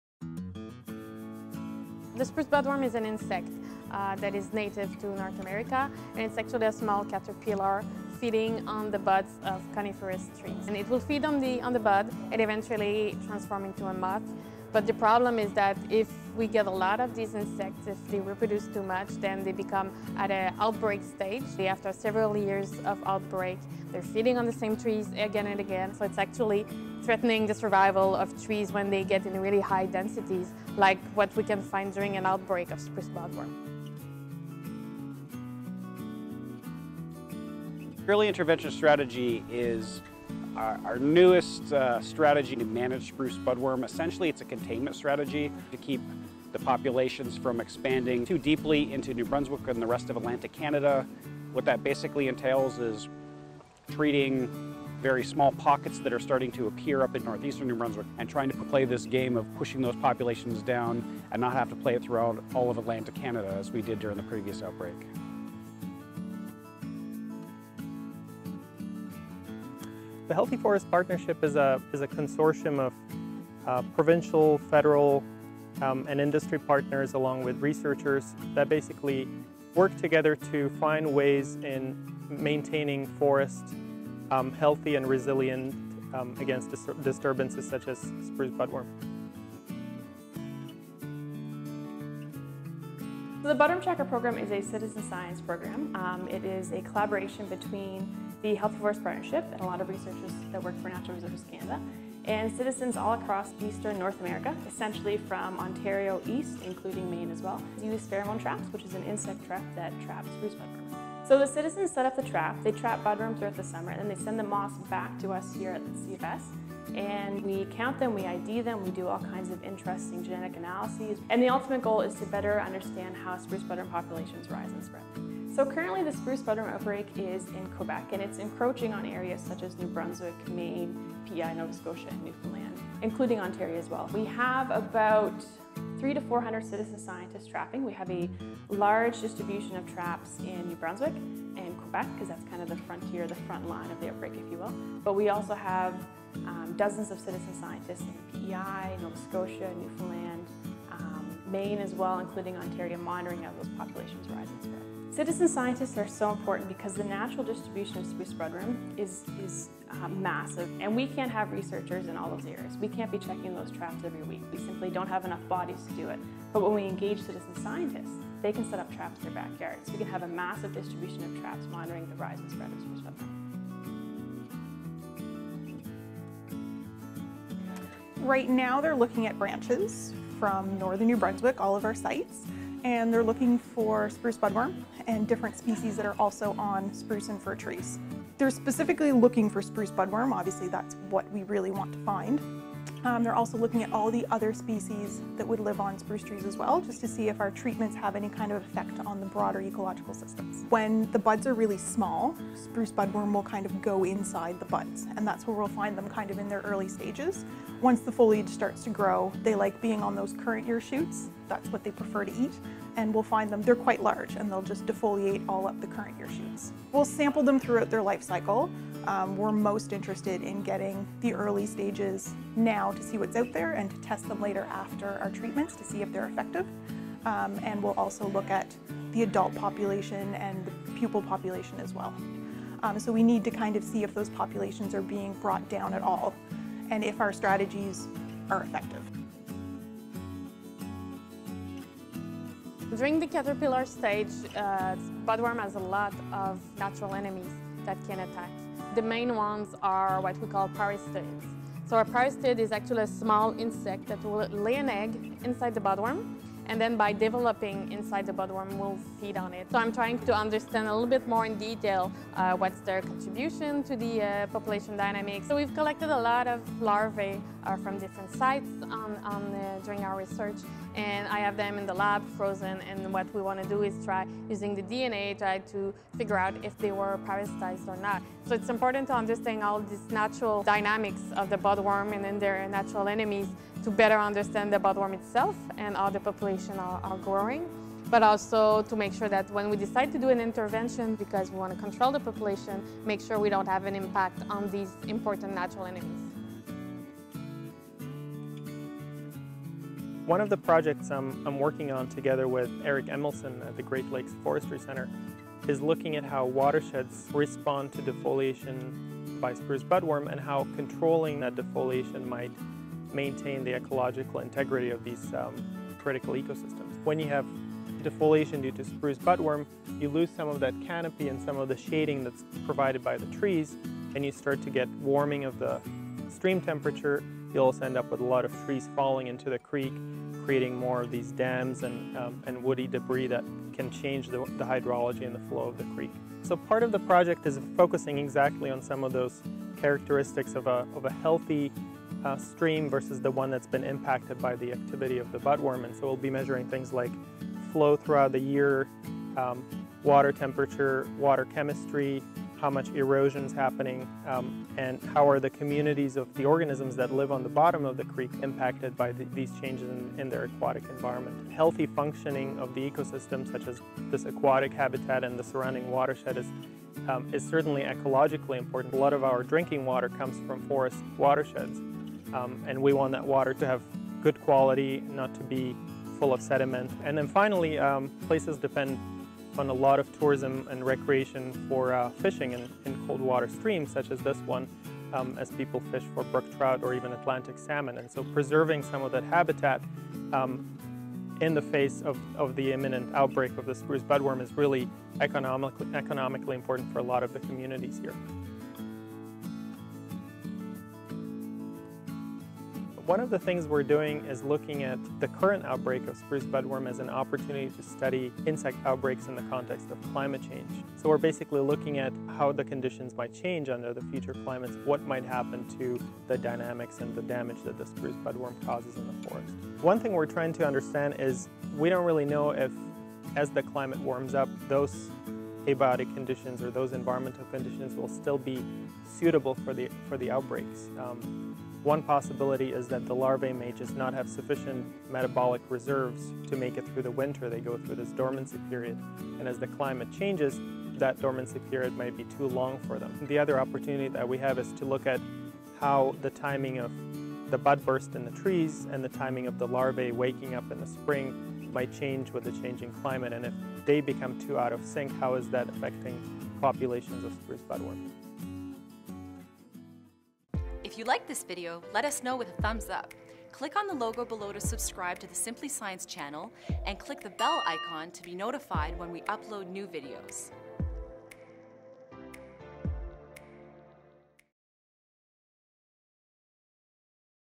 Slowing Down the Spruce Budworm
The spruce budworm is an insect that is native to North America, and it’s actually a small caterpillar feeding on the buds of coniferous trees. Learn about the strategies to study and manage the spruce budworm.